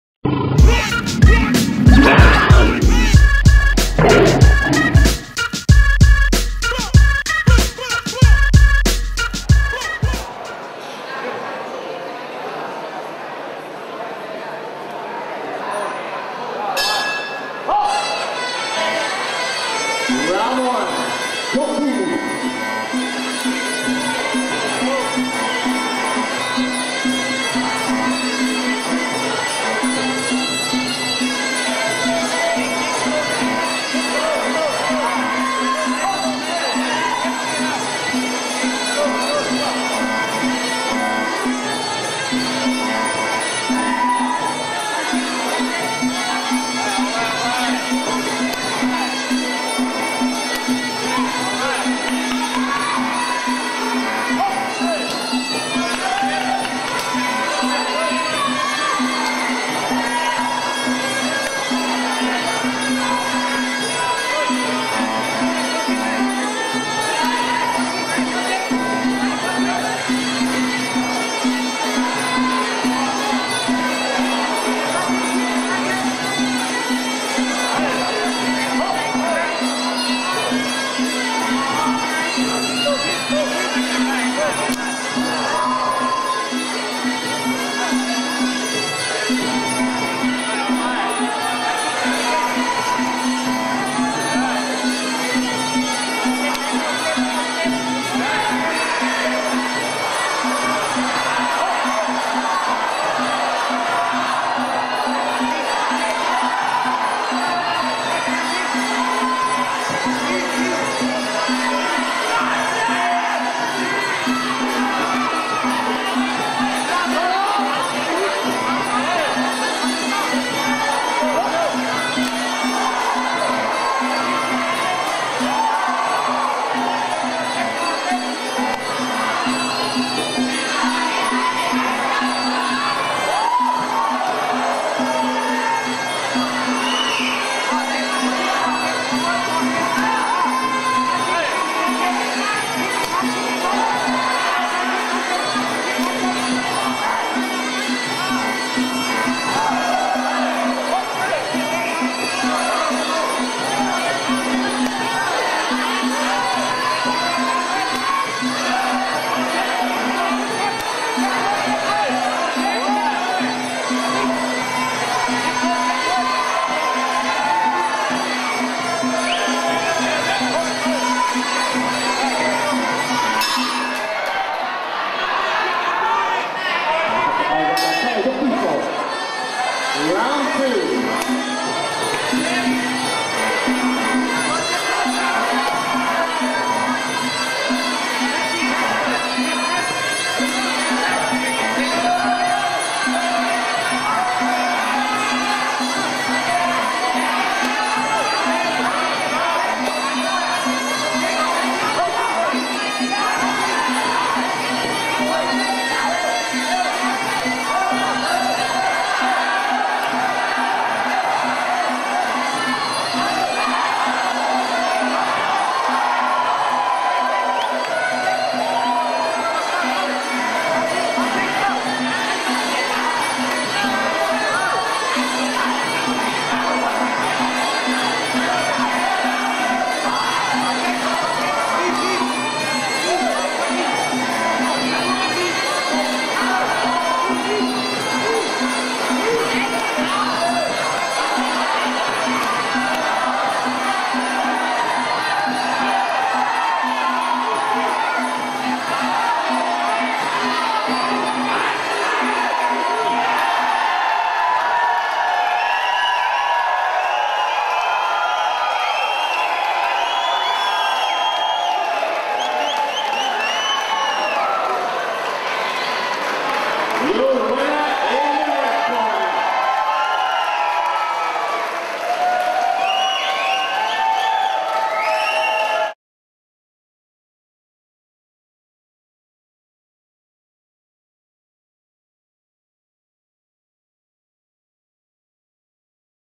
Nick (Tiger Muay Thai) vs. Fahlitkit (Thailand) @ Patong Thai Boxing Stadium
Nick fighting for Tiger Muay Thai & MMA Training Camp, Phuket, Thailand, scores a 2rd KO at Patong Thai Boxing Stadium on May 6, 2010.